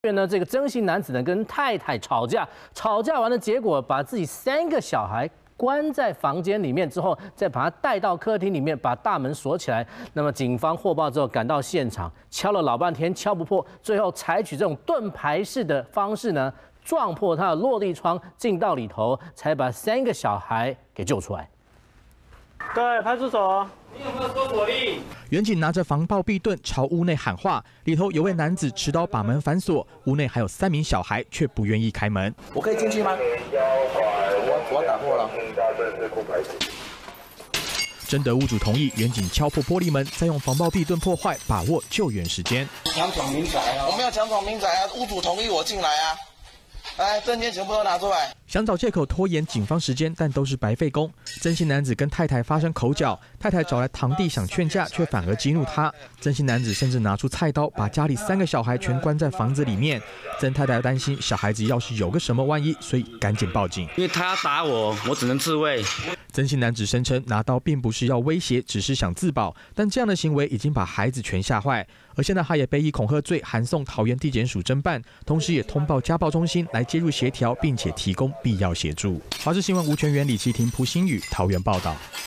夫妻口角持刀反鎖 警破門及時救人｜華視新聞 20201208
桃園這對夫妻吵架，竟然演變成持刀恐嚇的驚險場面，這名曾姓男子因為跟太太起口角，又不甘心對方找來堂弟助陣協調，不但持刀恐嚇，一氣之下，還把自己跟3個小孩反鎖在屋內。太太嚇的趕緊找員警來幫忙，員警使用「防暴臂盾」跟警棍敲破玻璃門才及時攔阻。